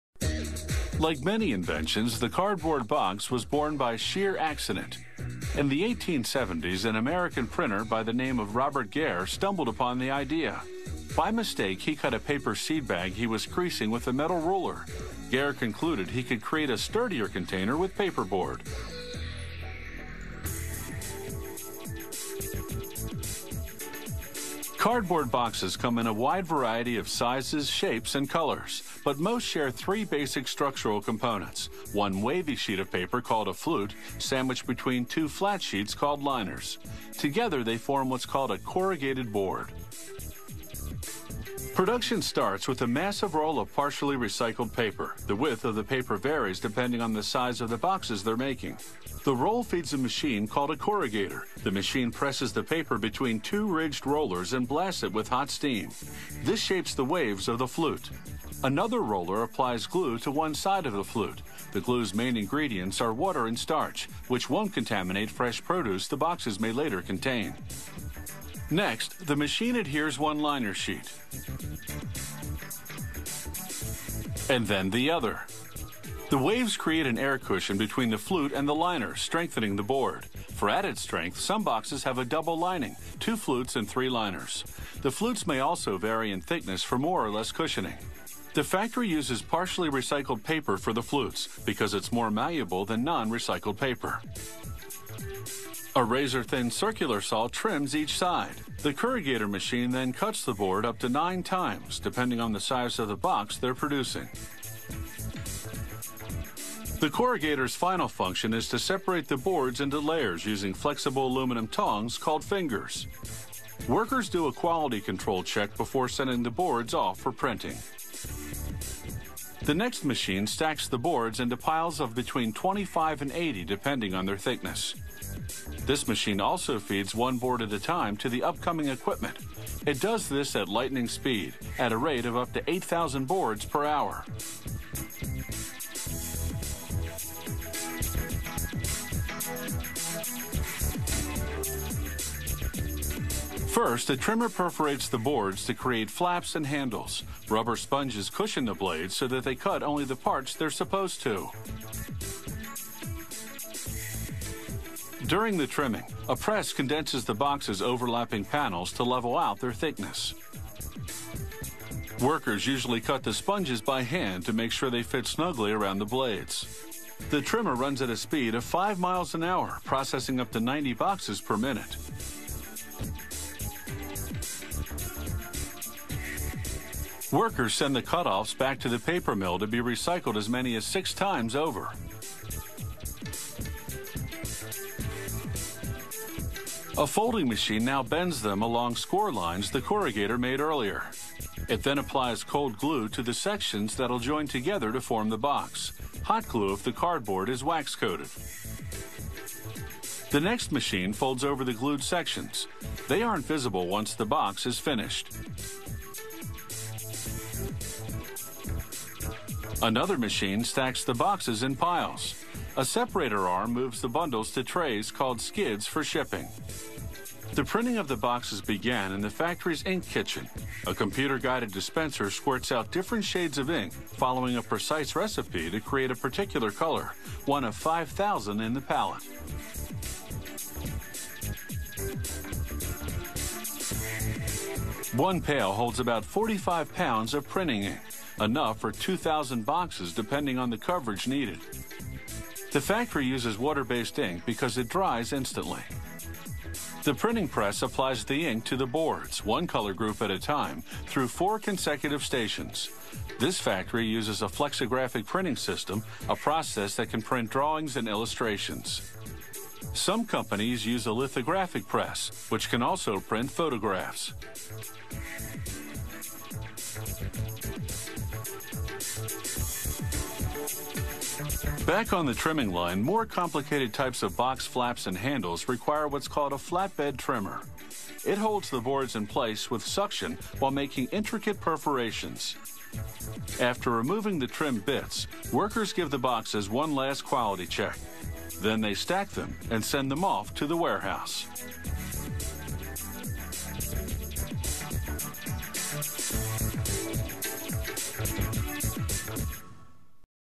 How are Cardboard Boxes Made
Like many inventions, the cardboard box was born by sheer accident. In the 1870s, an American printer named Robert Gair stumbled upon the idea. He cut a paper seed bag by mistake while creasing it with a metal ruler. Gair concluded he could create a sturdier container with paperboard. Now that was thinking outside the box. Cardboard boxes come in various sizes, shapes, and colors, but most share three basic structural components: one wavy sheet of paper called the flute sandwiched between two flat sheets called liners. Together, they form what's called the corrugated board. Production starts with a massive roll of partially recycled paper.

The width of the paper varies depending on the size of the boxes they're making. The roll feeds a machine called the corrugator. The machine presses the paper between two rigid rollers and blasts hot steam. This shapes the waves of the flute. Another roller applies glue to one side of the flute. The glue's main ingredients are water and starch, which won't contaminate the fresh produce the boxes may later contain. Next, the machine adheres one liner sheet and then the other. The waves create an air cushion between the flute and the liner, strengthening the board. Some boxes have a double lining, two flutes, and three liners for added strength. The flutes may also vary in thickness for more or less cushioning. The factory uses partially recycled fluted paper because it's more malleable than non-recycled paper. A razor-thin circular saw trims each side. The corrugator machine then cuts the board up to nine times, depending on the box size they're producing. The corrugator's final function is to separate the boards into layers using flexible aluminum tongs called fingers.

Workers do a quality control check before sending the boards off for printing. Depending on their thickness, the next machine stacks the boards into piles of between 25 and 80. This machine also feeds one board at a time to the upcoming equipment. It does this at lightning speed, at a rate of up to 8,000 boards per hour. First, a trimmer perforates the boards to create flaps and handles. Rubber sponges cushion the blades so that they cut only the parts they're supposed to. During the trimming, a press condenses the boxes' overlapping panels to level out their thickness. Workers cut the sponges by hand to ensure they fit snugly around the blades. The trimmer runs at a speed of 8 kilometers per hour, processing up to 90 boxes per minute.

Workers send the cutoffs back to the paper mill to be recycled as often as six times. A folding machine now bends them along score lines the corrugator made earlier. It then applies cold glue to the sections that will join together to form the box and hot glue if the cardboard is wax-coated. The next machine folds over the glued sections. They aren't visible once the box is finished. Another machine stacks the boxes in piles. The separator arm moves the bundles to trays called skids for shipping. The printing of the boxes began in the factory's ink kitchen. A computer-guided dispenser squirts out different shades of ink following a precise recipe to create a particular color, one of 5,000 in the palette. One pail holds about 20 kilograms of printing ink, enough for about 2,000 boxes, depending on the coverage needed. The factory uses water-based ink because it dries instantly. The printing press applies the ink to the boards one color group at a time through four consecutive stations. This factory uses a flexographic printing system, a process that can print drawings and illustrations. Some companies use a lithographic press, which can also print photographs. Back on the trimming line, more complicated types of box flaps and handles require what's called a flatbed trimmer. It holds the boards in place with suction while making intricate perforations. After removing the trimmed bits, workers give the boxes one last quality check, then stack them and send them to the warehouse.